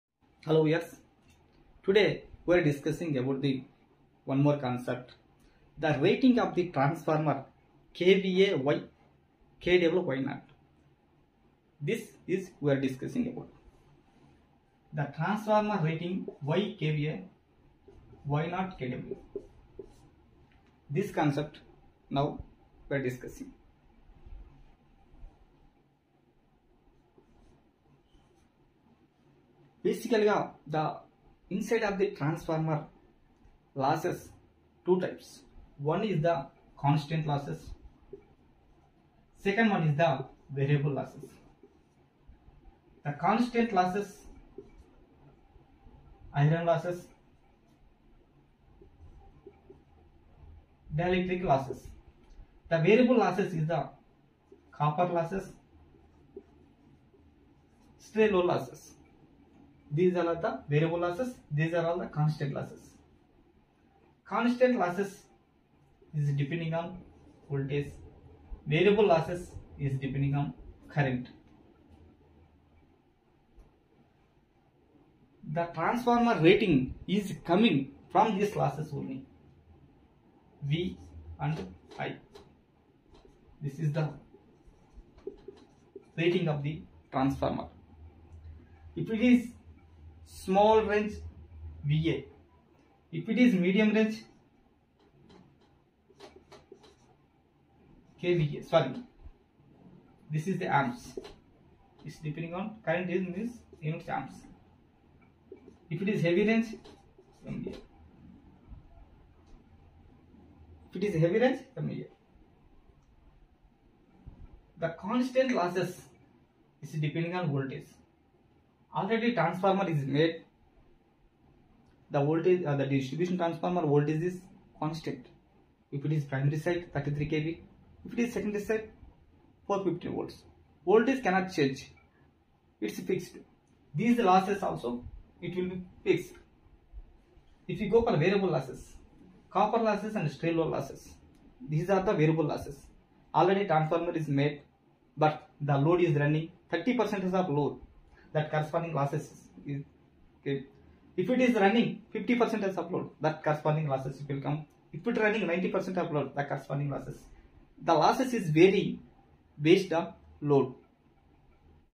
Transformer Rating KVA why in English//transformer rating kVA why
Hello guys, My Name is Ramireddy and I've Created this Channel to be Helping hand if Students in their Study.
Here you will get Lectures on Why Transformer Rating KVA //Transformer//Electrical Engineer.